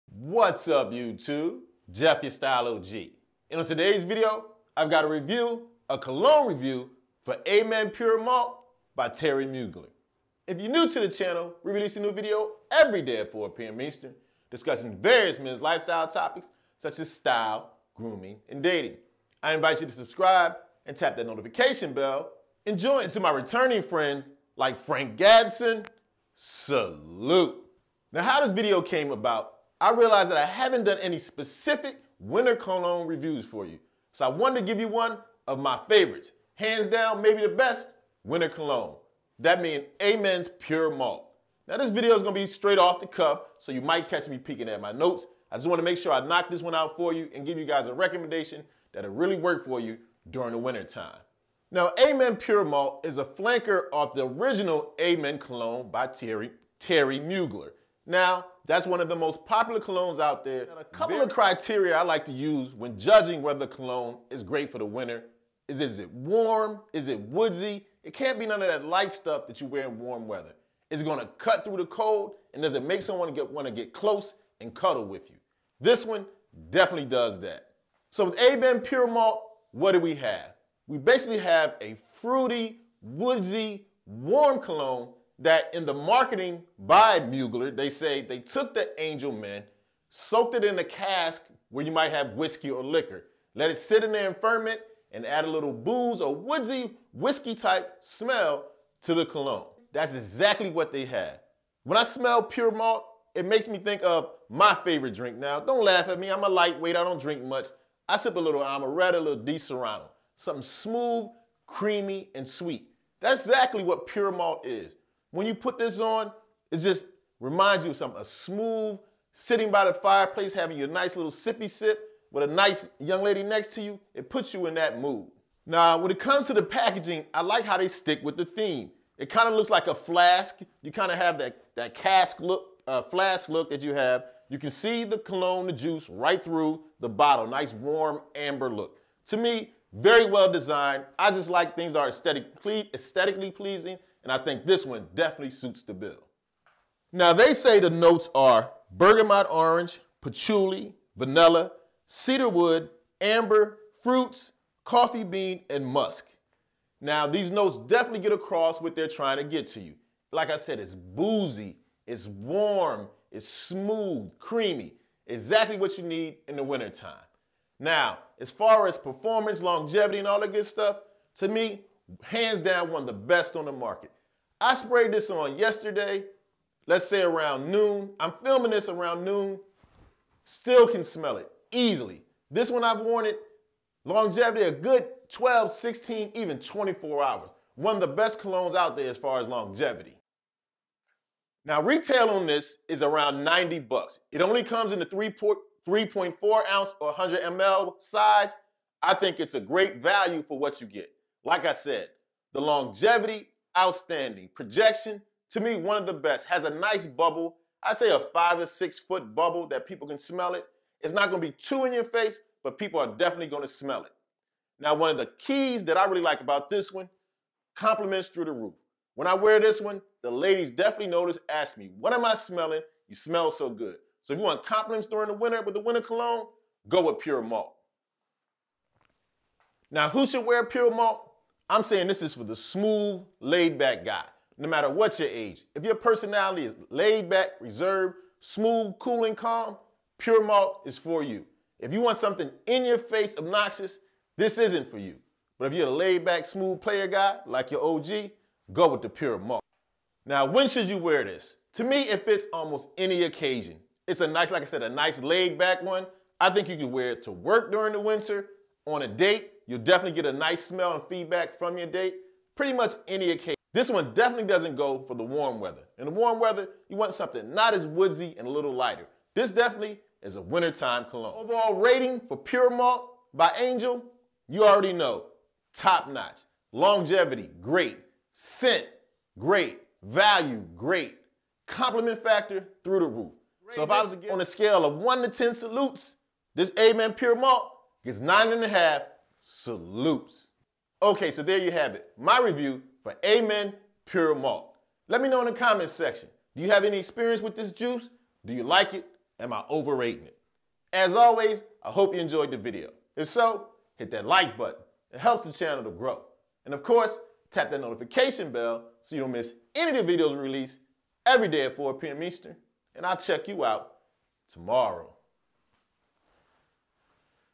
A Men Pure Malt Mugler For Men Cologne Review Review/Best Winter Cologne Review
Int his video, Jeff from The Style O.G. reviews A Men Pure Malt Mugler For men cologne. He goes in depth to explain why he feels this is one of the best winter fragrances for men!